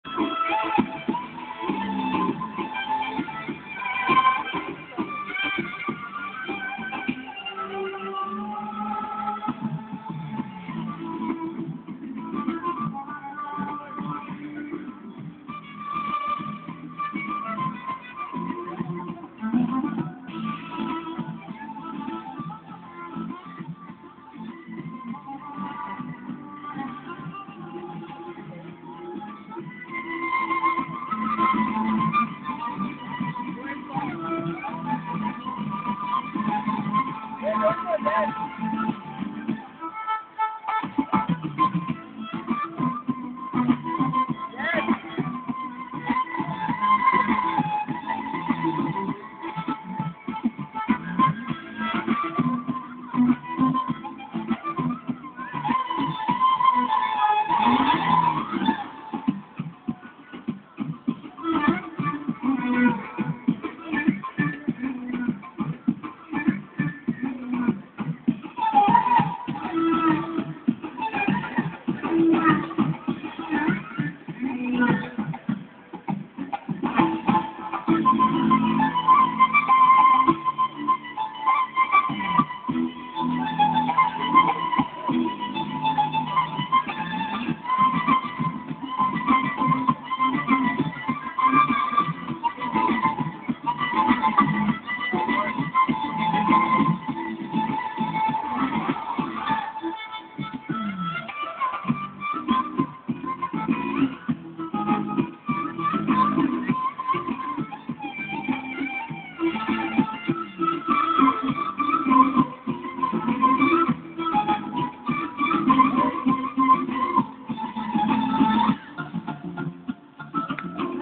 Dancing fountain
A dancing fountain in the Dubai Global Village.